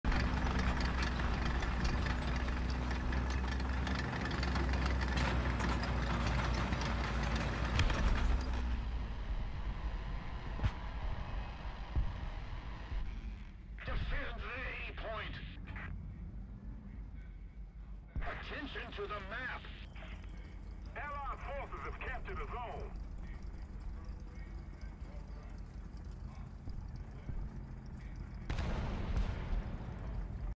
Anyone Notice These Megaphones?
Map is Vietnam.

TURN VOLUME UP ! ! !

What are they saying? Is it a real broadcast? 🤔